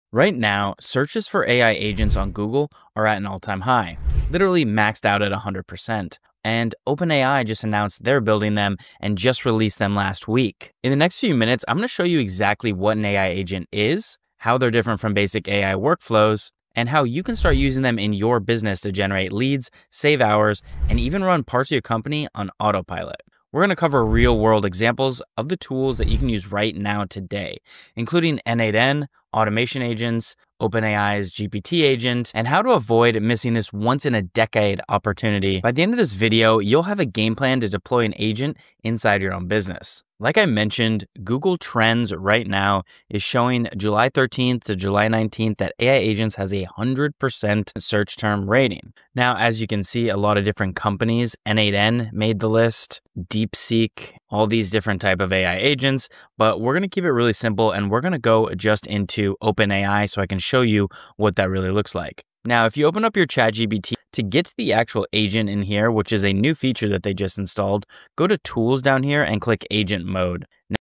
Can you afford to sleep on ai agents for your Business?
AI agents are exploding! See how OpenAI & N8N latest release can generate leads, save hours, and even autopilot parts of a business. Don't miss this game plan to deploy agents inside your company. Is this the future of work?